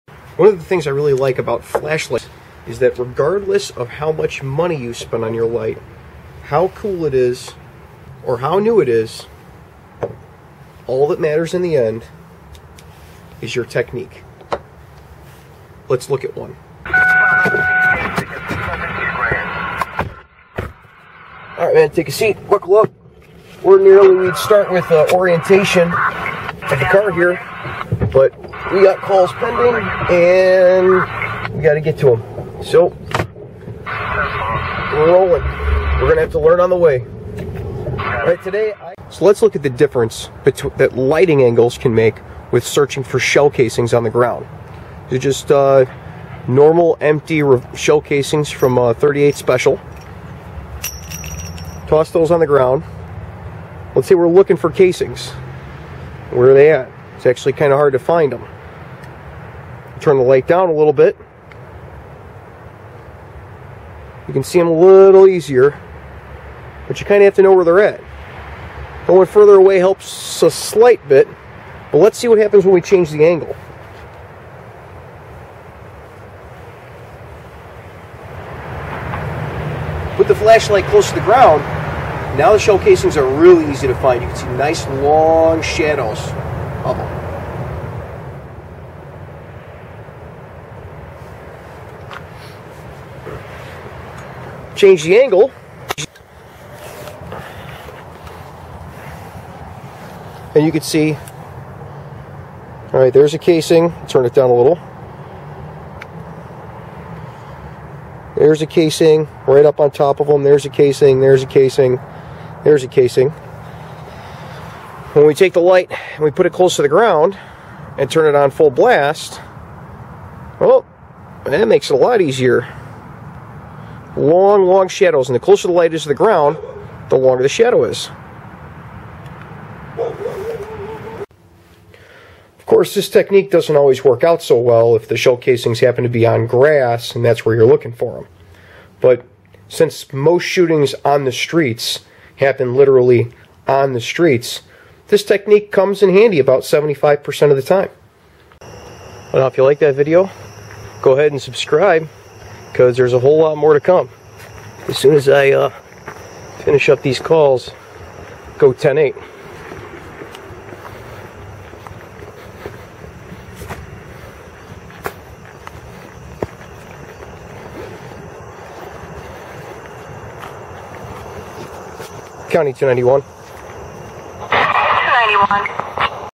Police Flashlight Technique: Finding Shell Casings
A fast, simple way to make crime scene processing a little bit easier at night. Find casings, and other small evidence, at night in a huge crime scene a little bit easier.

Inside a building or during the day Police work, especially handling crime scenes, is easier. At three AM when two slugs decide to shoot out the windows of moving cars at each other over a stretch of a couple city blocks, not so much. Add to this the limited manpower that most places can muster immediately and the other issues that have to be dealt with (crime scene security, interviews, supervision, call outs, photography, and collection amongst others) and resources start to get really thin.

You might be the only asset that is available to set the perimeter and determine the scope of the scene.

Featuring:

Streamlight stinger LED

Spent Buffalo bore .44 magnum shell casings.

   What could be simpler than using a flashlight (torch)? Well, it turns out that plenty of things could be.

  Hotly debated in the firearm training industry, every internet expert has a super secret squirrel USSOCOM/SEAL/DELTA/Ranger/SAS method of using a flashlight with a gun

   But, what about the everyday "non tactical" use of a flashlight? there is far more to it than the lay man might think...